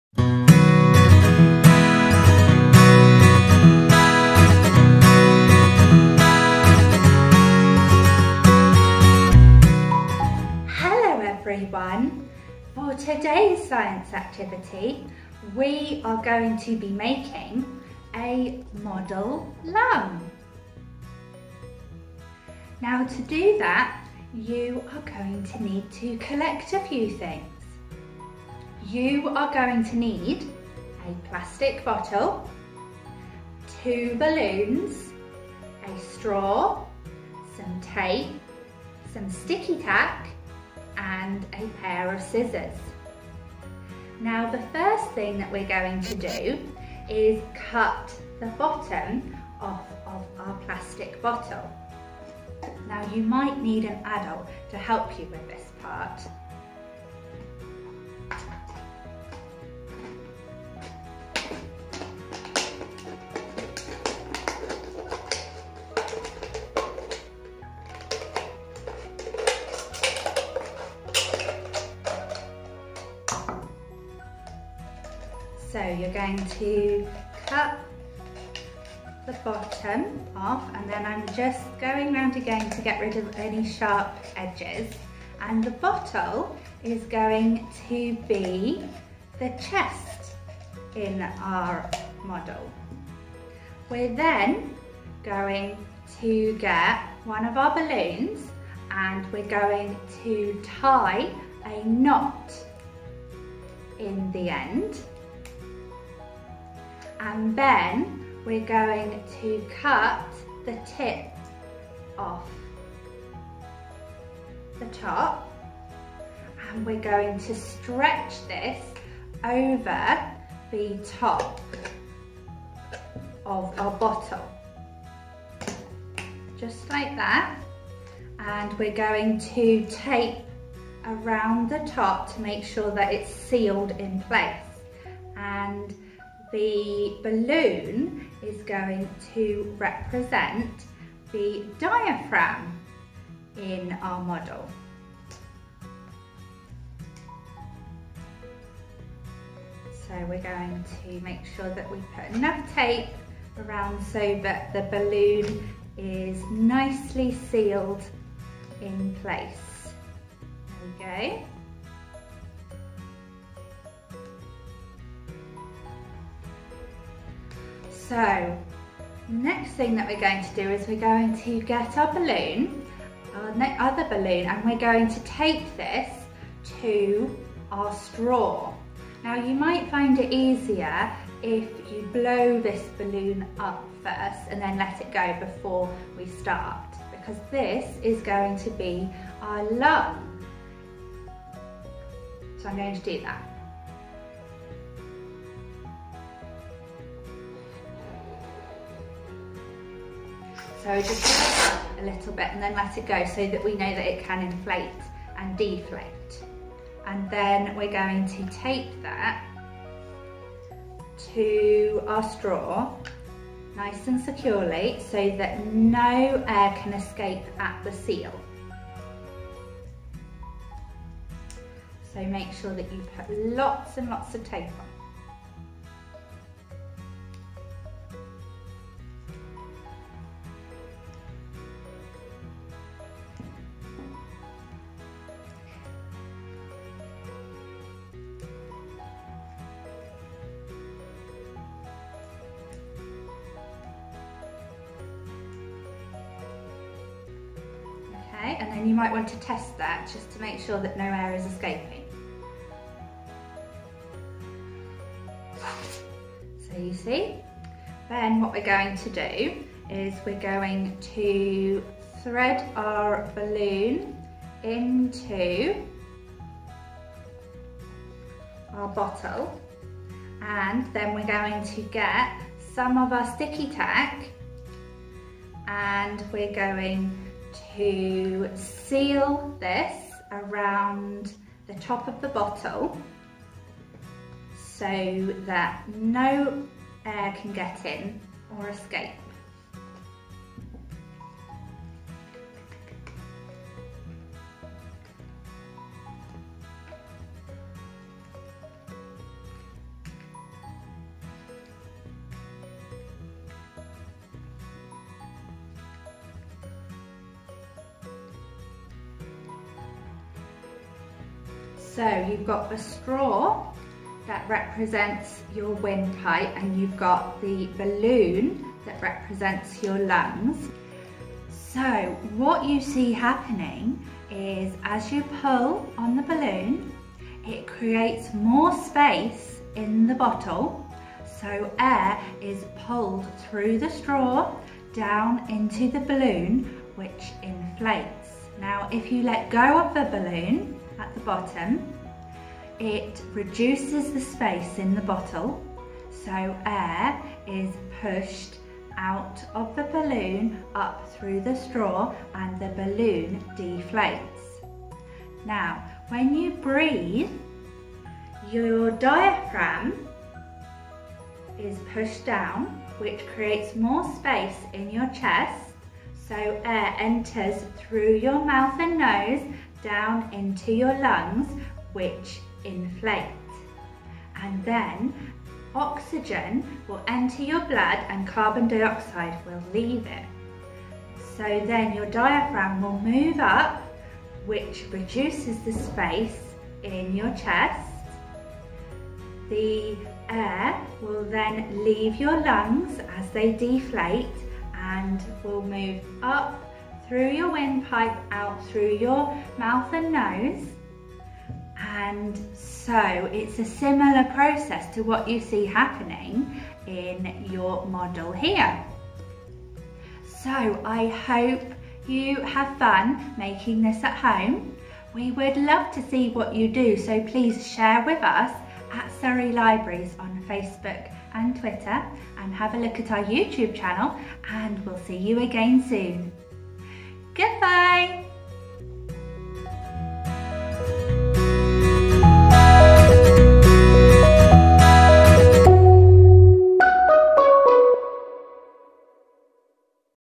Surrey Libraries STEM Science Club - Model Lung
The lungs are part of the respiratory system but you need more than just the lungs to breathe. Let’s find out more with Laura in today’s STEM activity.

You will need:
 - A plastic bottle
 - 2 balloons
 - Tape
 - Sticky tack
 - A straw
 - Scissors